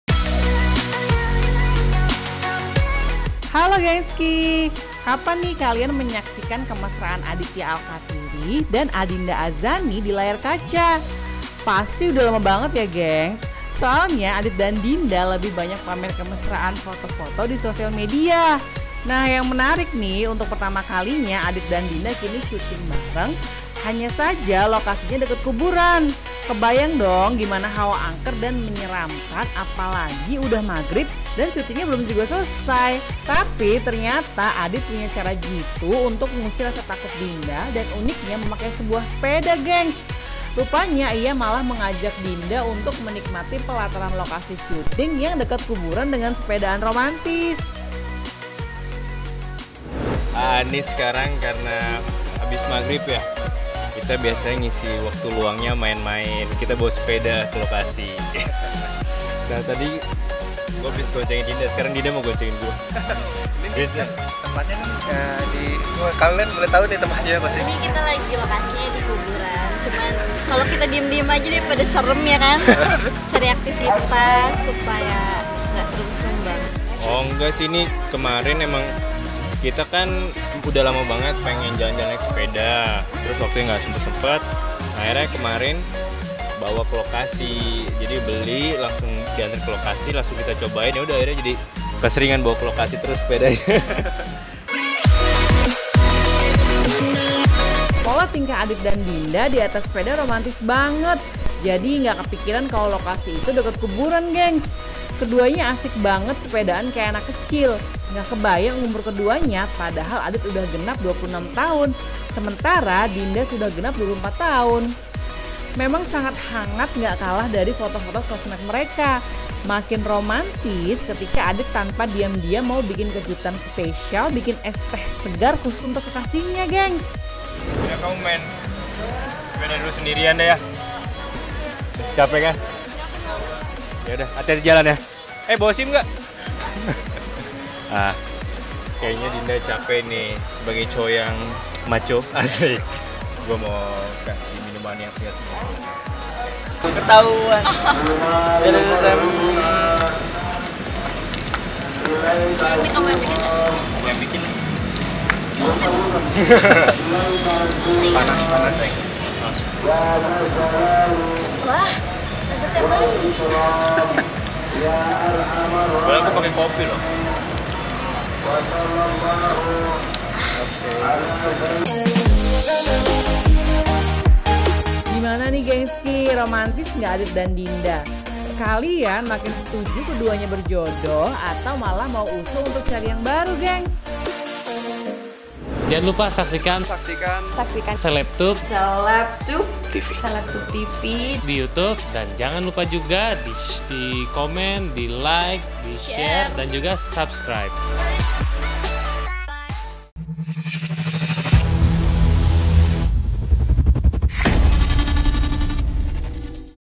SYUTING DEKAT KUBURAN, ADHITYA ALKATIRI DAN ADINDA AZANIUSIR HAWA ANGKER PAKAI ROMANTISME
Hello Gengskii. !!

KAPAN KALIAN MENYAKSIKAN KEMESRAAN ADHITYA ALKATIRI DAN ADINDA AZANI DI LAYAR KACA?/ PASTI SUDAH LAMA BANGET GENGS/ SOALNYA ADIT DAN DINDA LEBIH BANYAK PAMER KEMESRAAN FOTO-FOTO DI SOSIAL MEDIA// NAH YANG MENARIK UNTUK PERTAMAKALINYA ADIT DAN DINDA KINI SYUTING BARENG HANYA SAJA LOKASINYA DEKAT KUBURAN